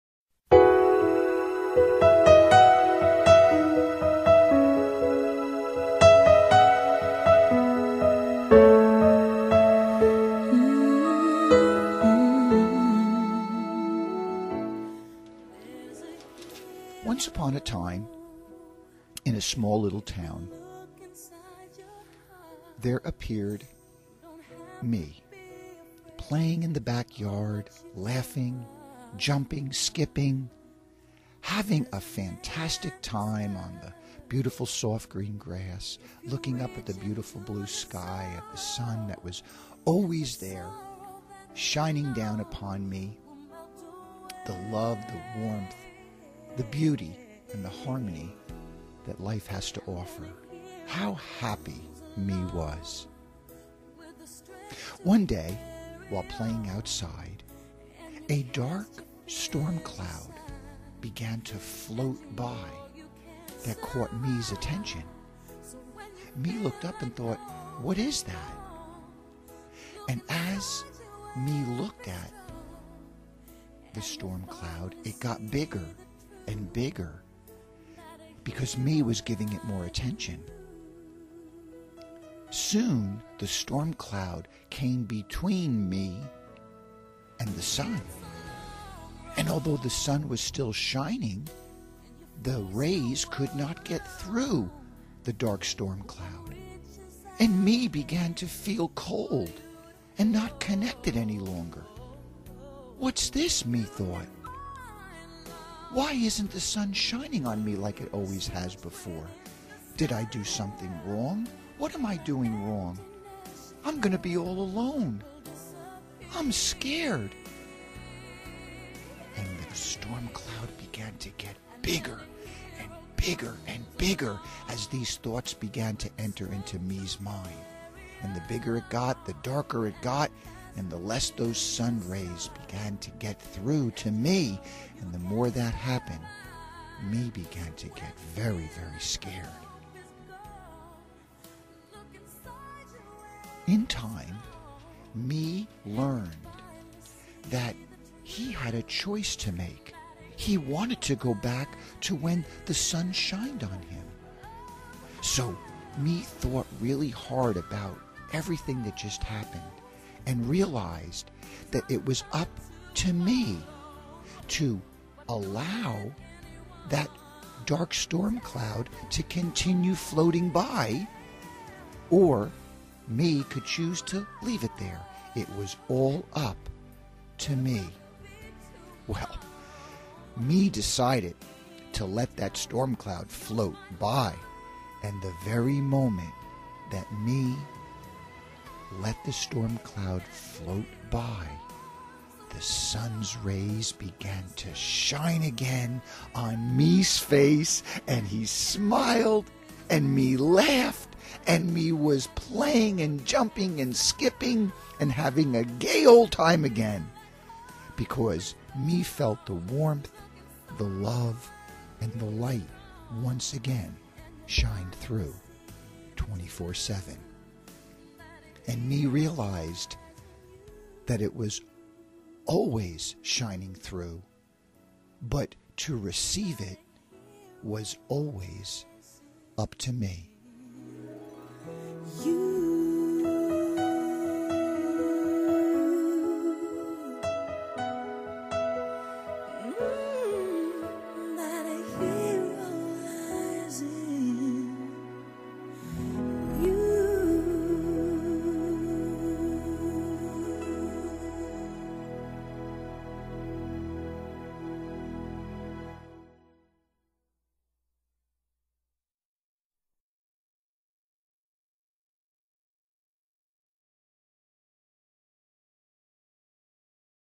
Loving Fear with The Love and Light within, a true LIFE hero story By Paul Agliata
Me and My Sun is a true life story by Paul Agliata.  Me is you and you are me and we are all together. We come from the same source energy and if your life was viewed from the source energy side love and well being would always be flowing and shining to you. The choice is up to you to receive it and send it back.  Live to give. I invite you to look at your own life and see if a hero lives in you.